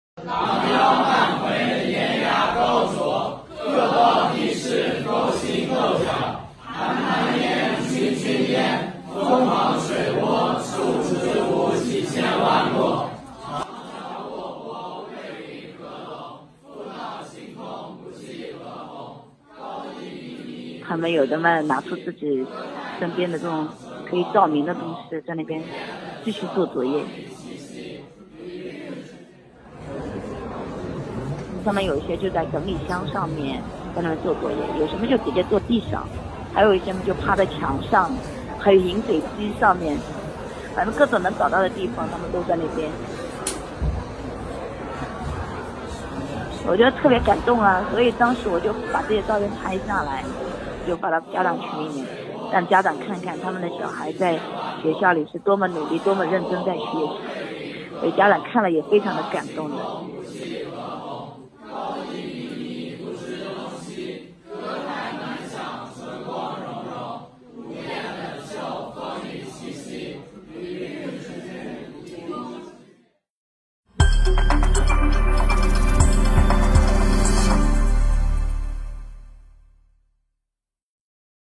浙江杭州一学校教室停电，高中生黑暗中齐声背古文
社会新闻 每日更新